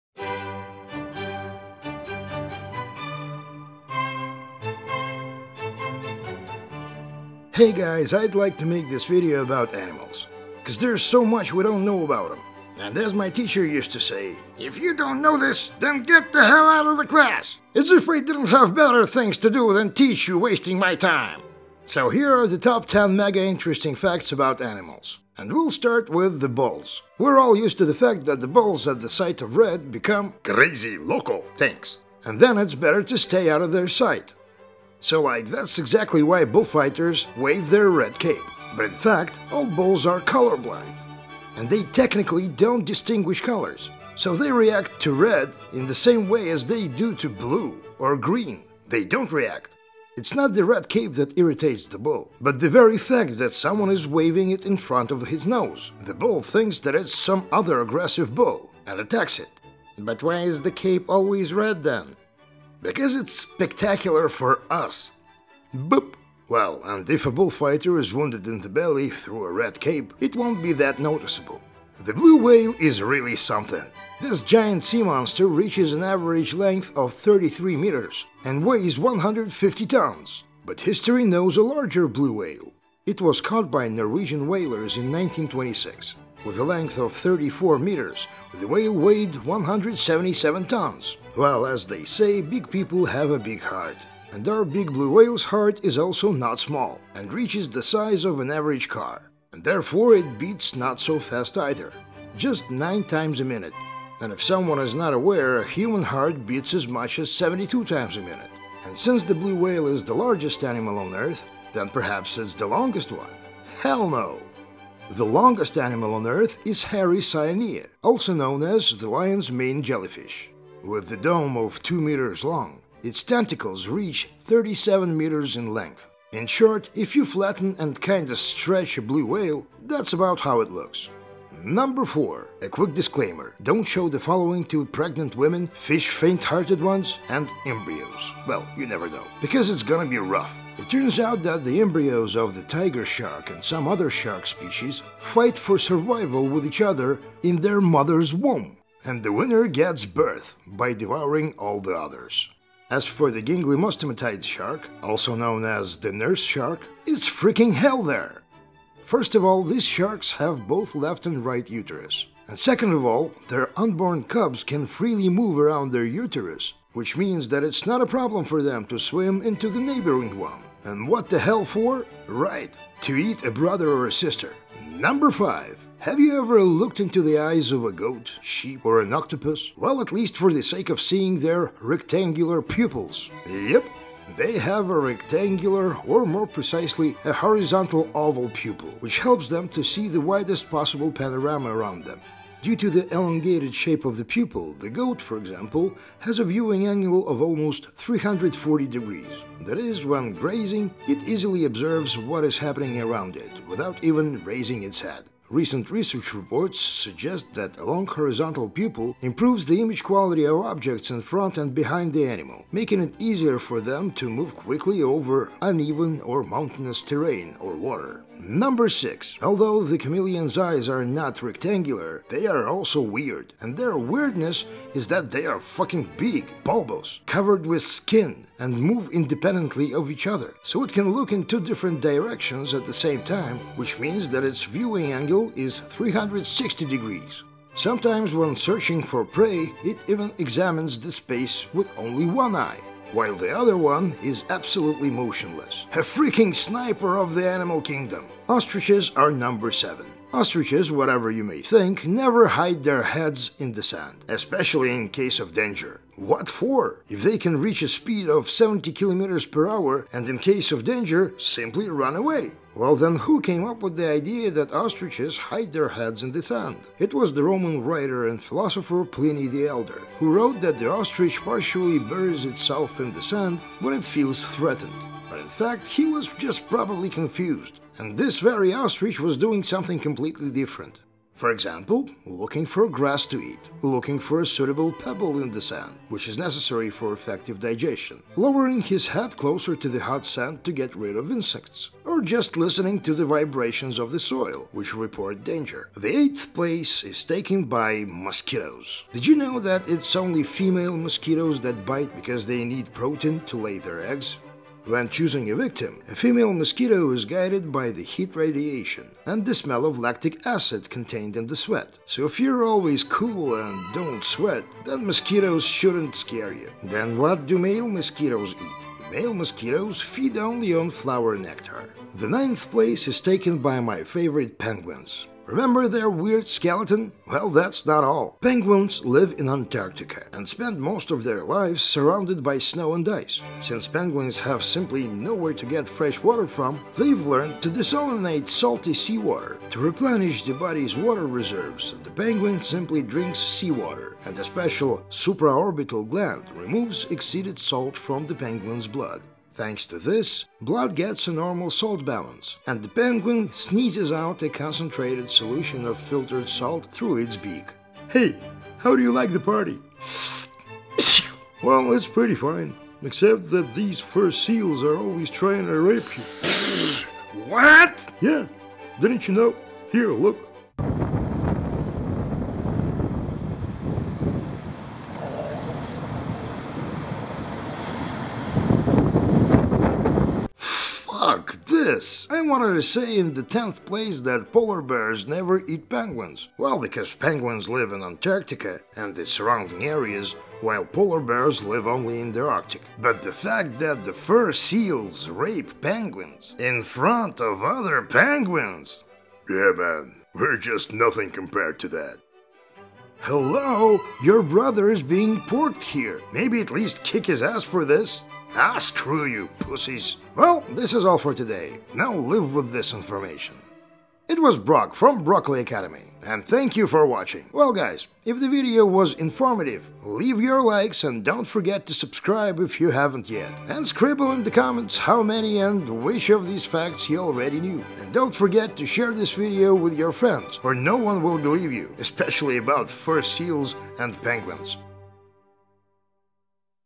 Top 10 Interesting Facts You`ve Newer Known about Animals - Broccoli Academy
Did you miss Sam O'Nella Academy? Here are new animated facts from its successor Broccoli Academy! Do bulls hate the color red? What is the biggest animal in the world? Why do penguins sneeze? BONUS FACT IN THE VIDEO: Fur seals have been caught engaging in an extreme form of lovemaking behavior. Why do penguins hate it so much? Hilariously useful facts, trivia cartoony characters, funny stories from the depths of the world history.